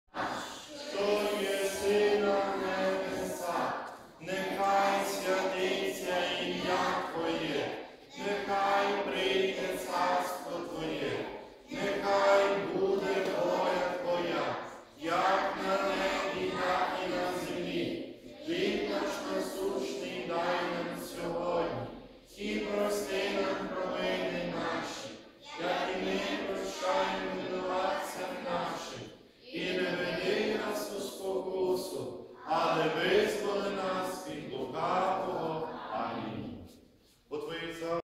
Свята Літургія для дітей у храмі
"Веселі канікули з Богом" на парафії Покрови Пресвятої Богородиці, Тернопіль, серпень 2015.